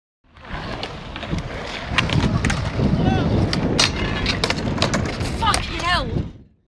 Smith bail
me doing a smith then bailing...